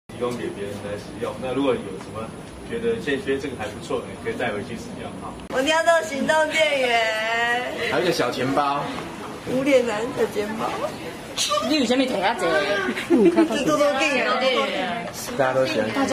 弘光科大成立惜福物資交換站 落實物資再利用
學生遺失物品常無人認領，弘光科技大學學務處成立「惜福物資交換站」，將學生拾獲、超過半年無人認領的物資陳列出來，供學生免費選用，師生也可將自己不用的東西整理出來，提供他人取用，推廣及落實「物資再利用、減少資源浪費」觀念，學生表示，到交換站可享受尋寶的樂趣，也可讓自己用不到的東西找到新主人，一舉多得。